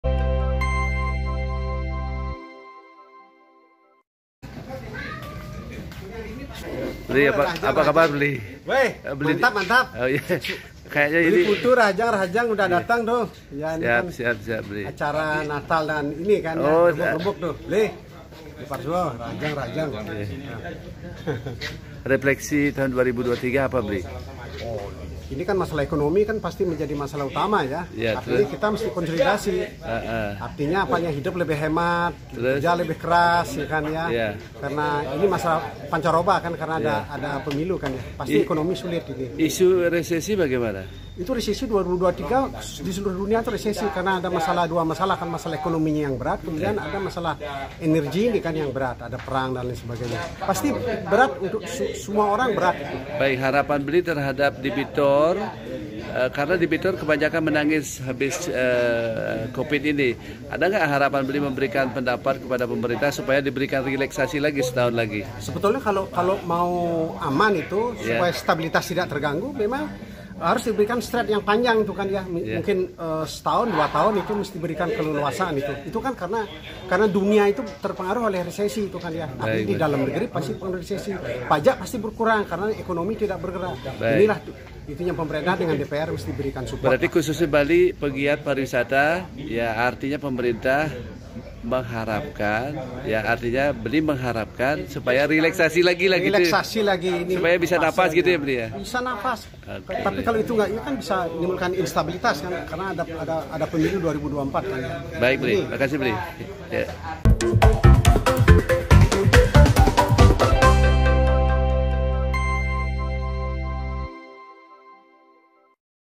Bagaimana Tanggapan Tentang Isu Resesi 2023?
Bertemu dengan Bapak Putu Suasta. Begini Tanggapan tentang isu resesi 2023